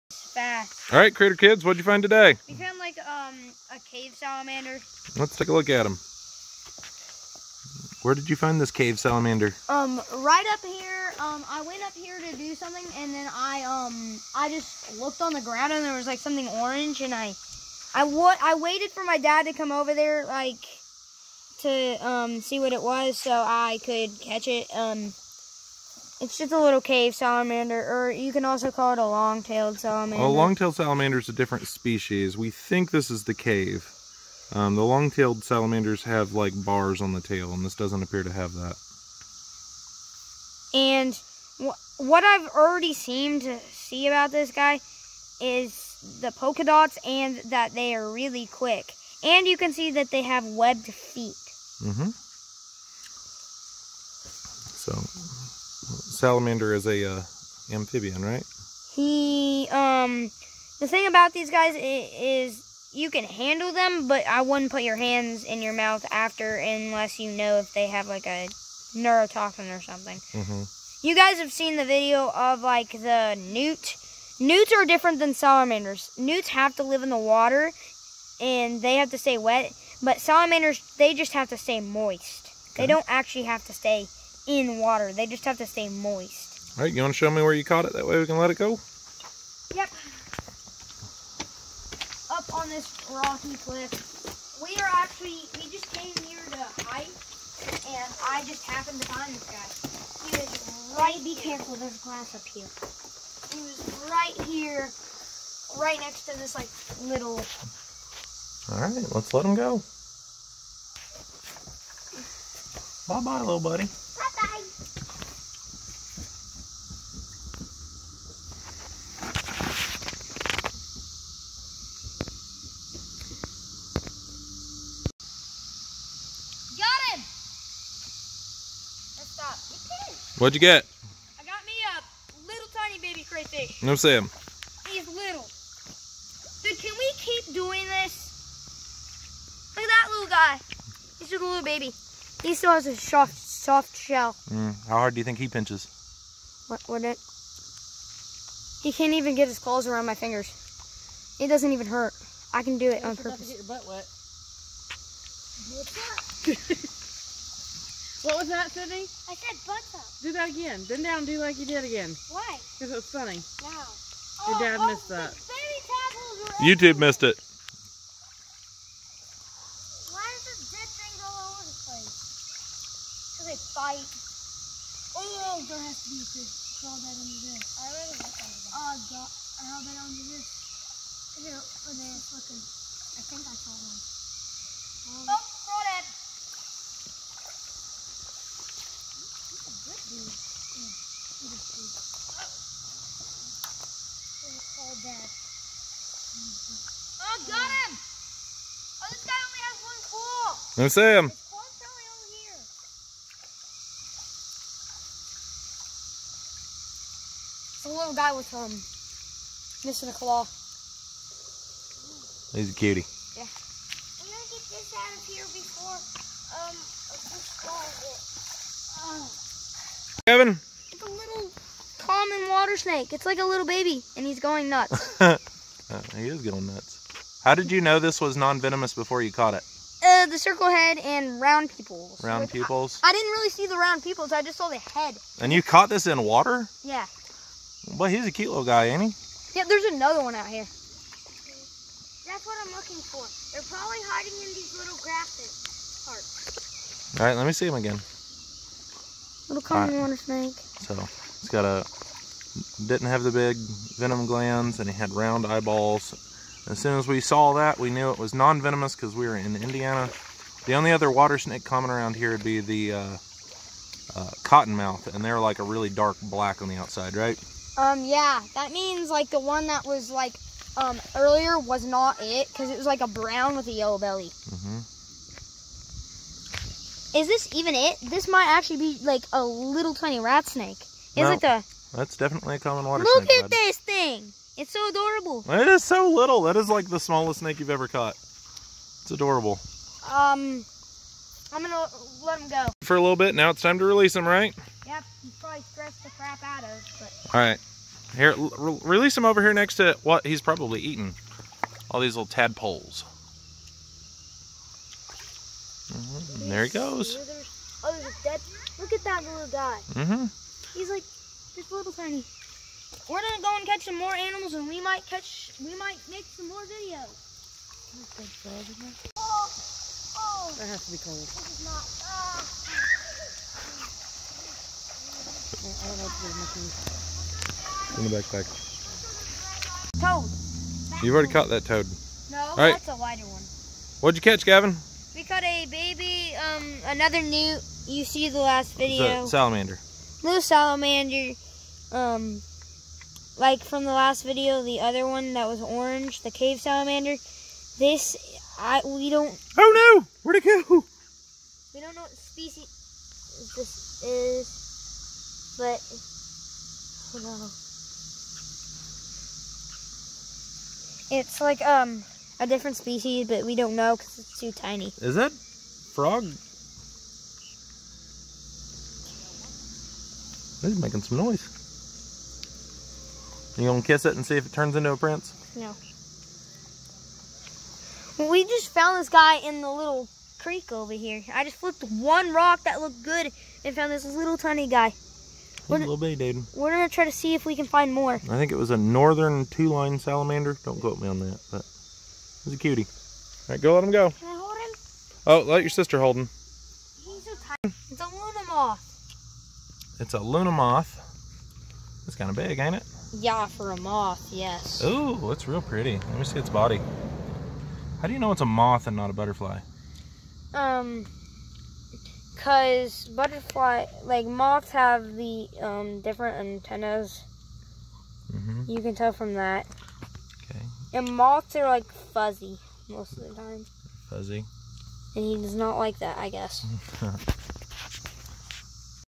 The Wildlife of Muskatatuck Park in North Vernon Indiana
For Gavin's 10th birthday we decided to go to Muscatatuck Park in North Vernon Indiana. It is a wonderful place to go for a hike. Today however, we never got to take a hike because we kept getting so distracted by all the wonderful wildlife.